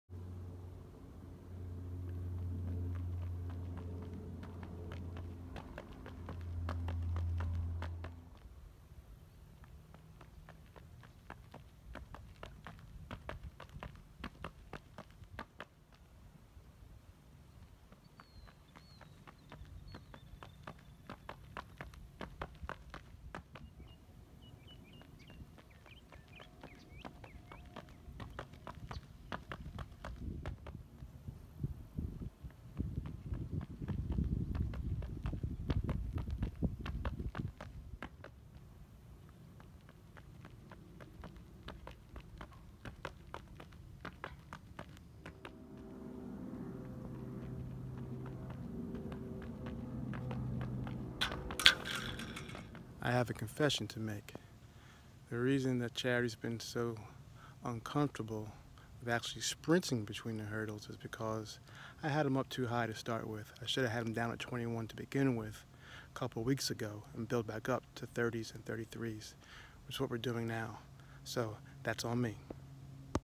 Charity 092219
Had a really good session with Charity Williams tonight. To help her get past the fear of the obstacles, caused by a couple falls last spring, I lowered the hurdles all the way down to 21”. Finally, for the first time this fall, she was aggressive in attacking the hurdles. We moved them up to 24”, and ended at 27”, and she never lost her aggression. I think it’s fair to say that I had underestimated how much the falls had gotten into her head, since they had happened in low-speed drills. So that was my mistake, which I acknowledge at the end of the video.